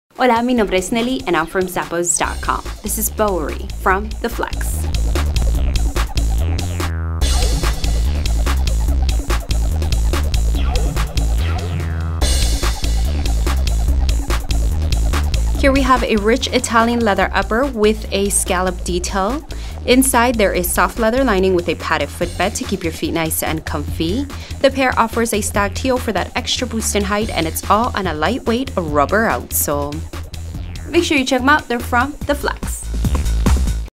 The FLEXX Bowery SKU: 9171221
This FLEXX&reg; Bowery Loafer is both classic and updated for versatile day-to-day wear.
Rich Italian leather upper with scalloped detailing.
Easy slip-on design.
Pointed toe silhouette.
Leather lining and insole.
Stacked heel.
Rubber outsole.
Imported.